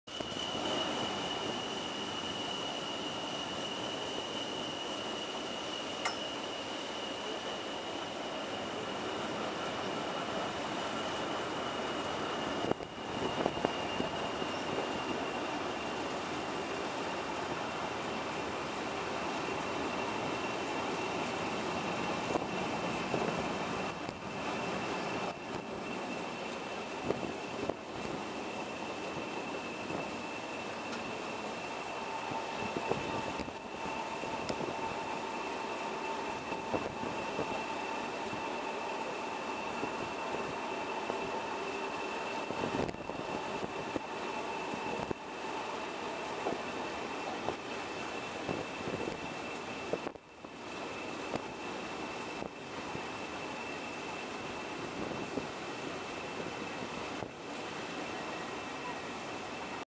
40145 12th June 2010 part 1 (Chester)
40145 approaches Chester on BLS railtour